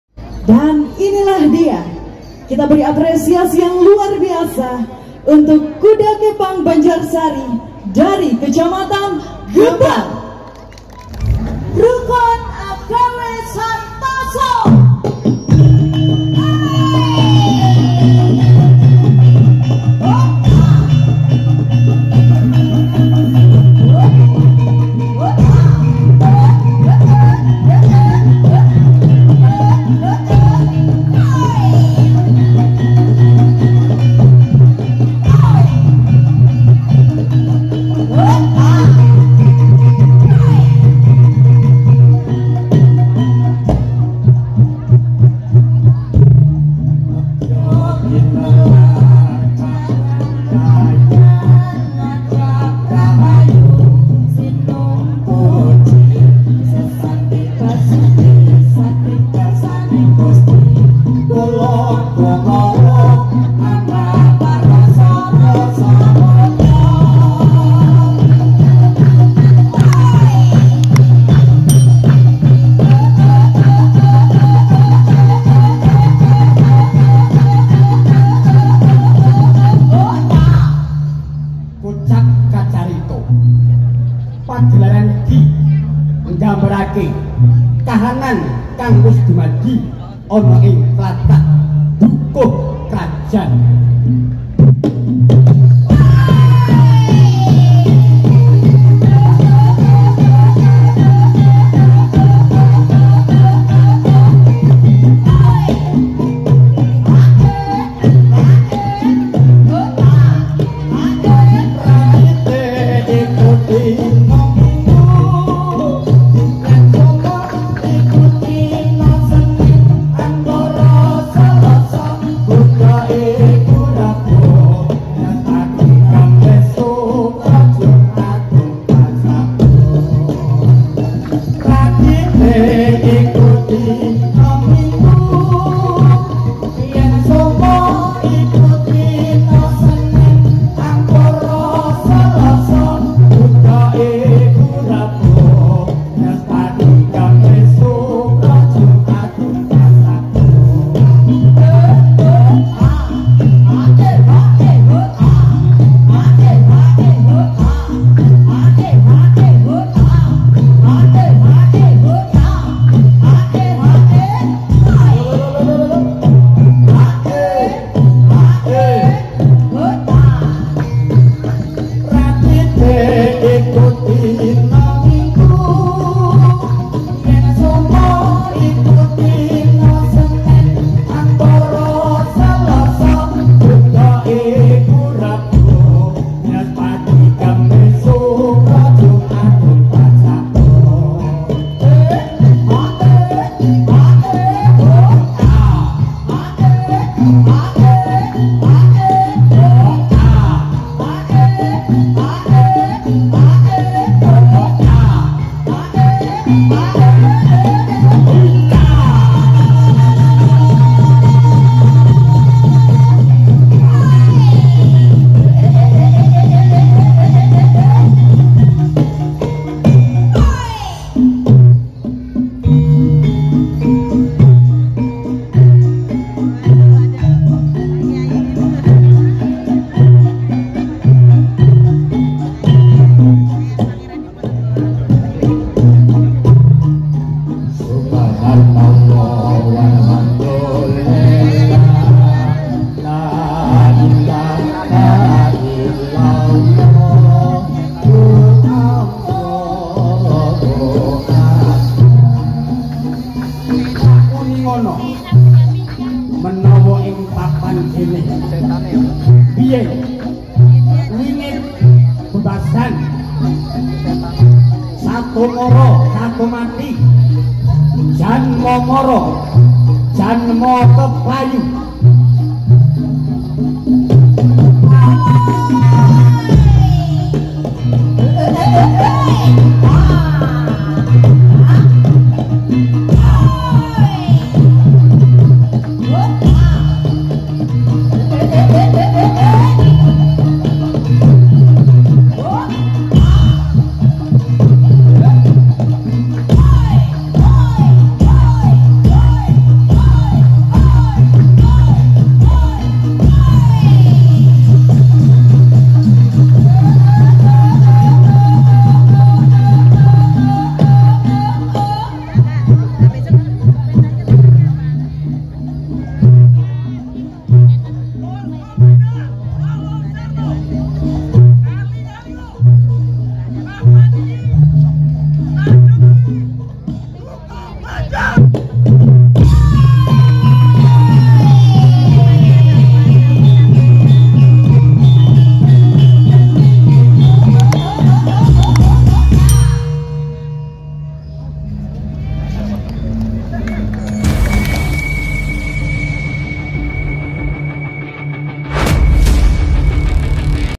Grebeg Budaya 2023 || Dalam Rangka Hari Jadi Purworejo Ke-192 || Duta Seni Kec.Gebang
☆Duta Seni Kec.Gebang☆
Grebeg Budaya 2023 rangkaian acara Hari Jadi Ke-192 Kabupaten Purworejo di ikuti duta Seni dari 16 Kecamatan yang ada di Kabupaten Purworejo.